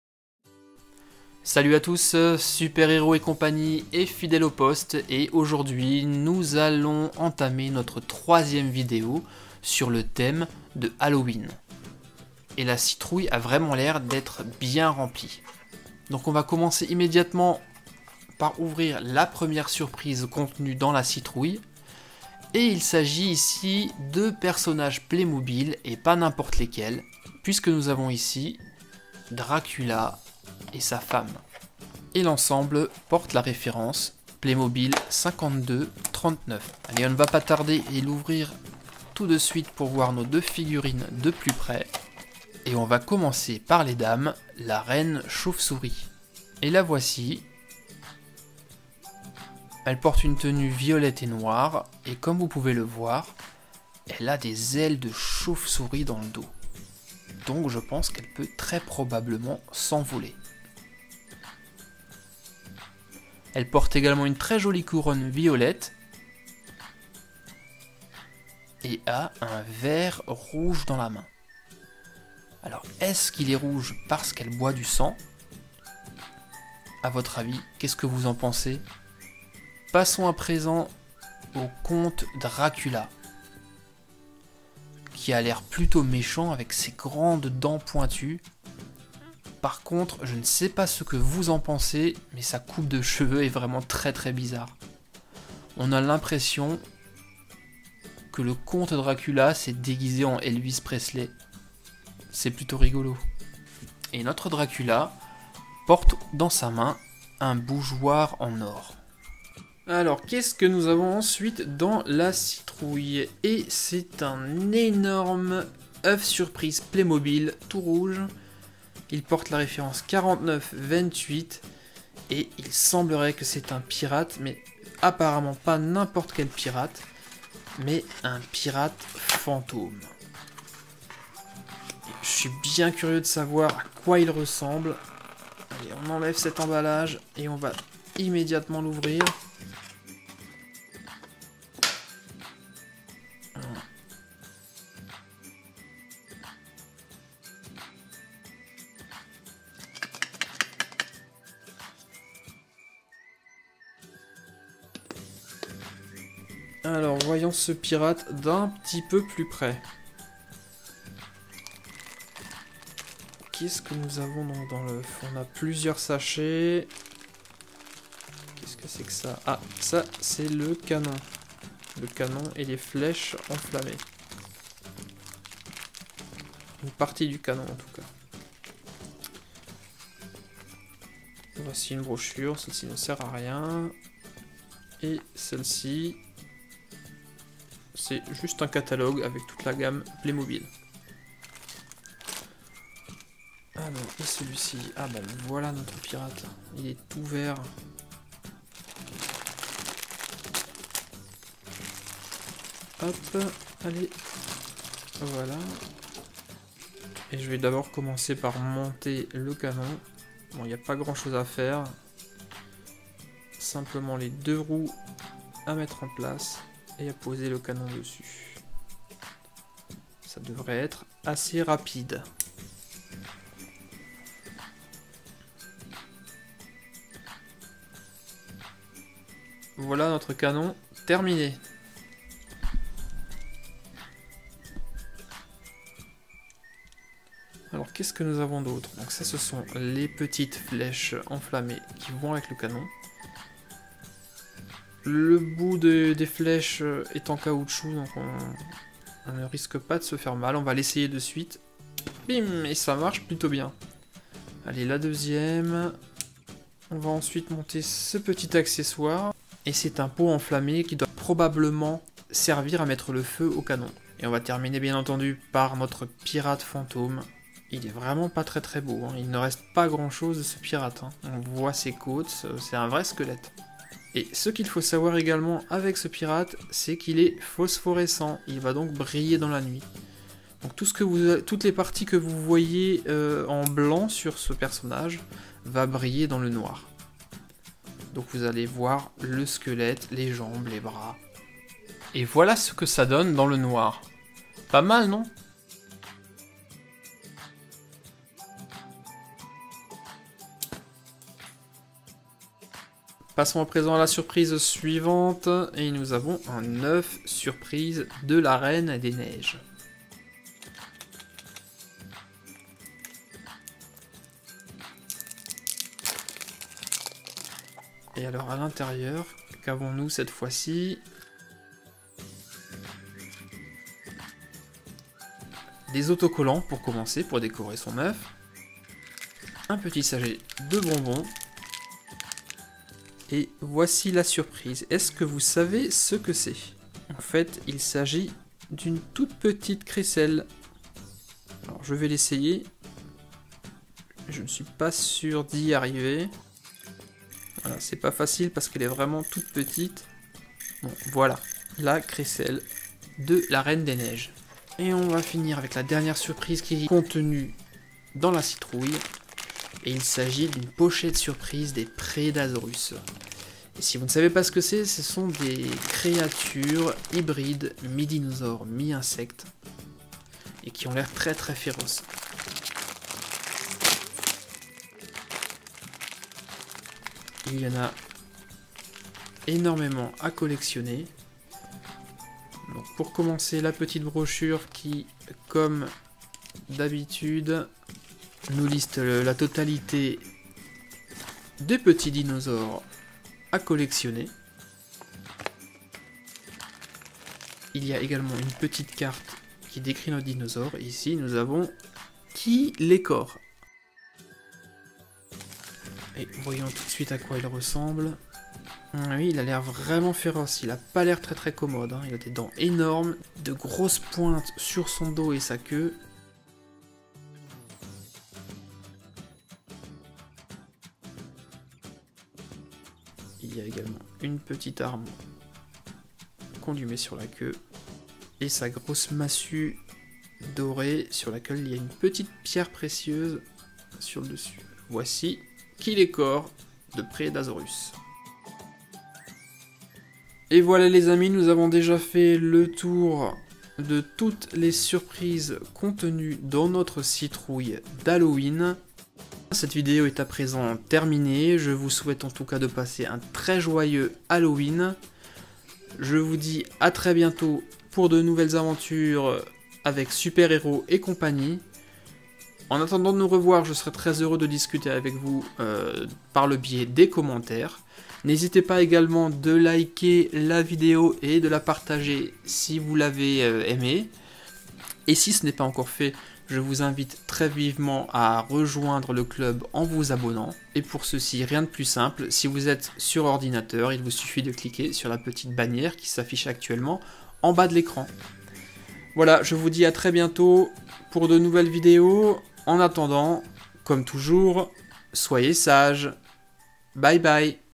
Jouet Halloween Playmobil Dracula Pirate Fantôme oeuf et pochette surprise français
SuperHerosEtCompagnie vous propose une nouvelle vidéo sur le thème de Halloween.
Notre citrouille est aujourd'hui remplie surprises, nous trouverons par exemple des Playmobil (5239 et 4928), des pochettes surprises et des oeufs.

video halloween qui fait peur